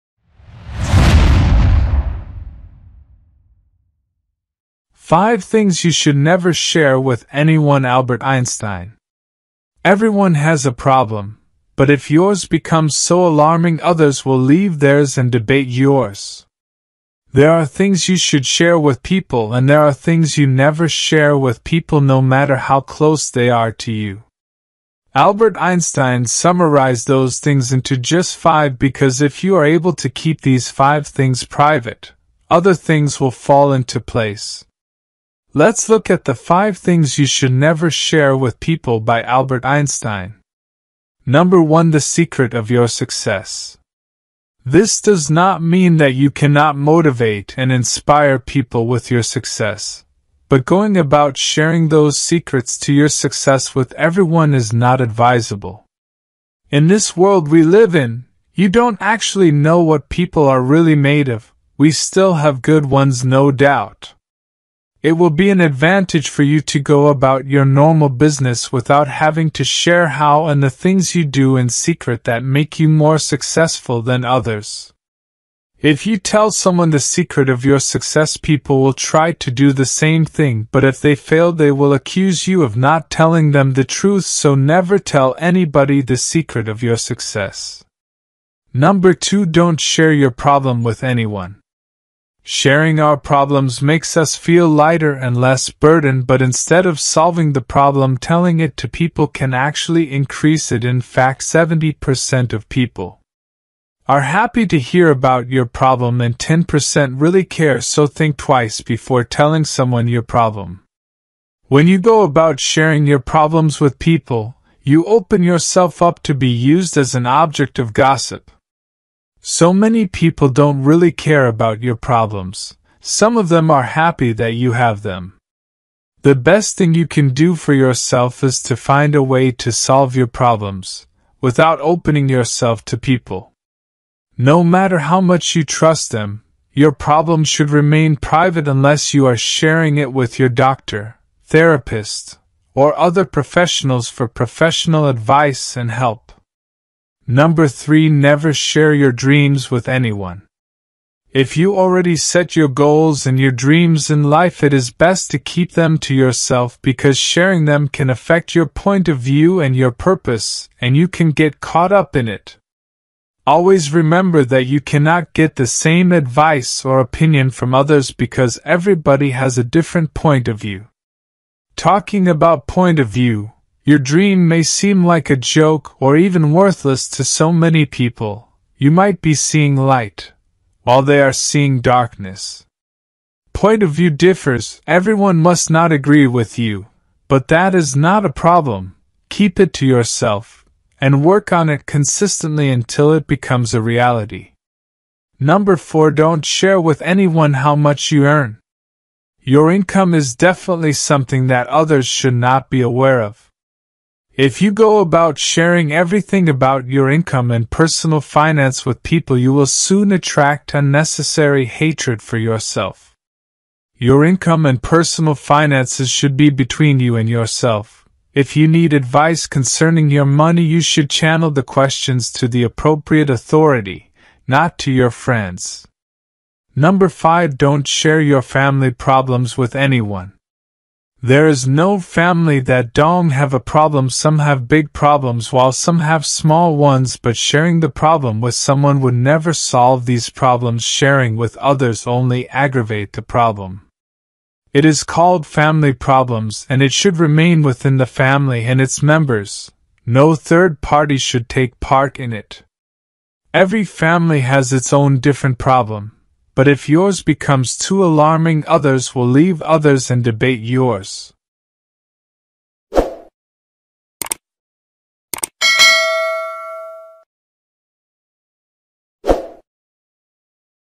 5 Things Never Share With Anyone | Albert Einstein Quotes| Learn With Quotes
5 Things Never Share With Anyone | Albert Einstein Quotes | Quotes Einstein|

Albert Einstein was a theoretical physicist born in Germany who is widely regarded as one of the greatest physicists of all time. Albert Einstein is best known for developing the theory of relativity, but he also made significant contributions to the development of quantum mechanics.

We hope you enjoyed this Wise Life Quotes by Albert Einstein a lot of work has put into it.